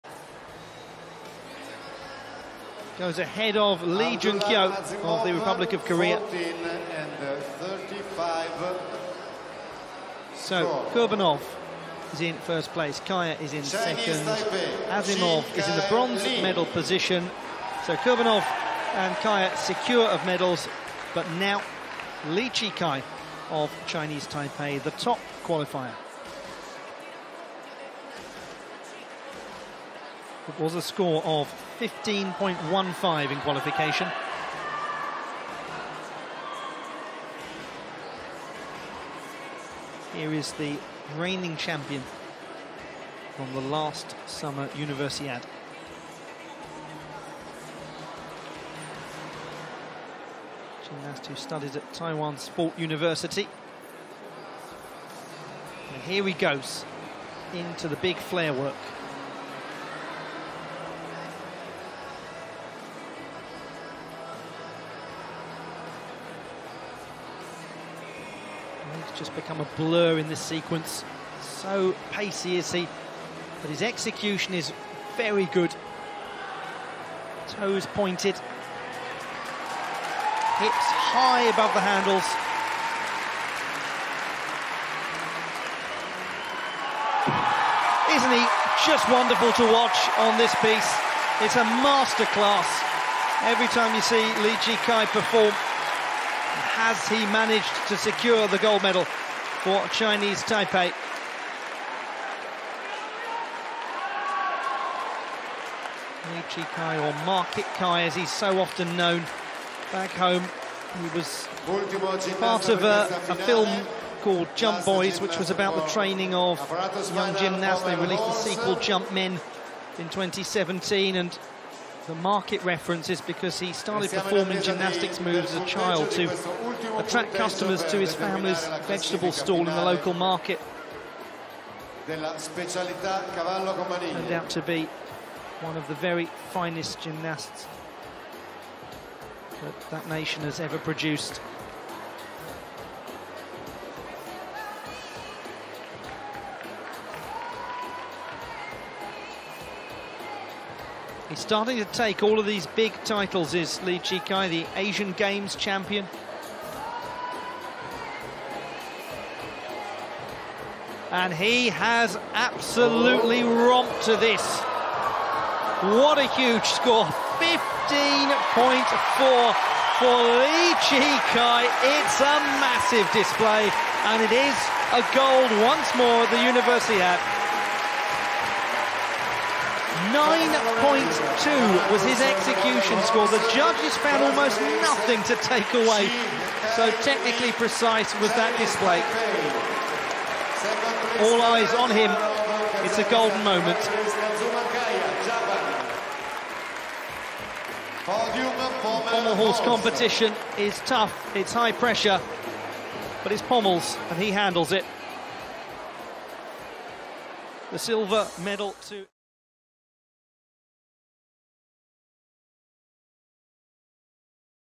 ::金牌 李智凱 LEE Chih-Kai :: 15.400超高分！競技體操 男子個人鞍馬2019拿坡里世大運 Summer Universiade
突破極限！二連霸 李智凱 在個人鞍馬項目獲得15.400超高分！！突破個人上屆15.3分最佳成績，也為代表團再度獲得一面金牌，請大家觀看精彩的影片～